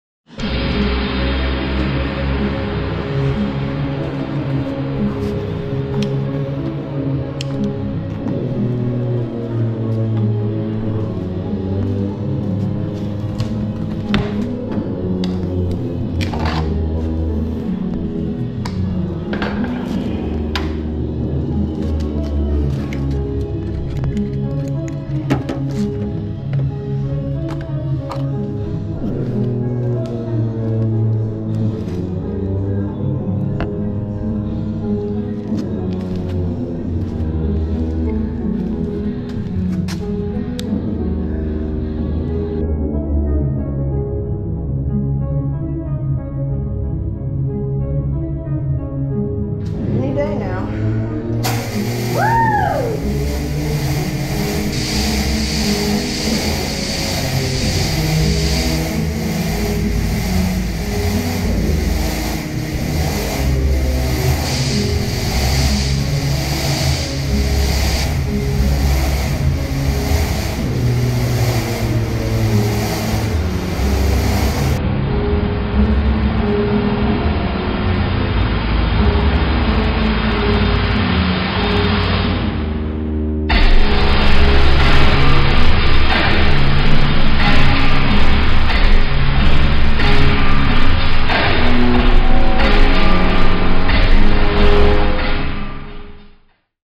Croc on a Bat | Action figure photography
Put a bunch of frozen water jugs in a tote. Fog machine hits the ice blocks and makes it stay low to the floor. Made an action scene with Waylon on the batmobile and he seems to be trying to rip the Bat out of his ride.